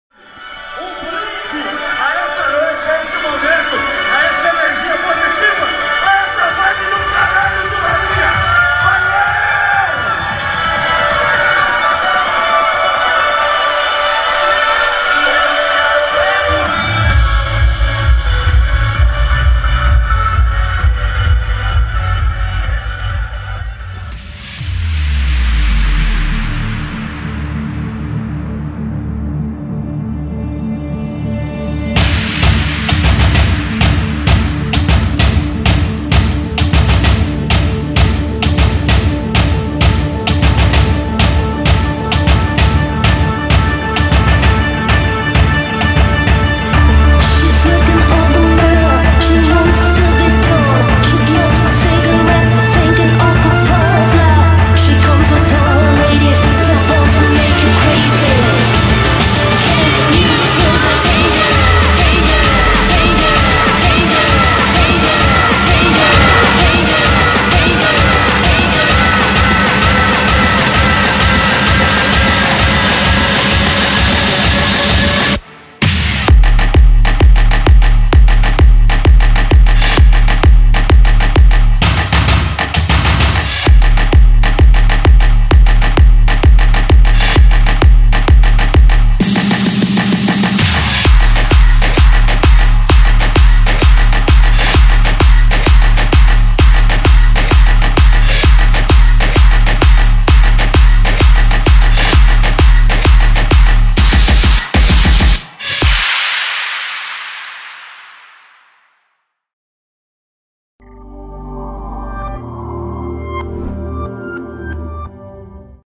Rock da House @ Heroes #2 Aftermovie
AFTERMOVIE da festa HEROES # 2 q rolou no dia 25/01/2014 na UNIK CLUB.

lml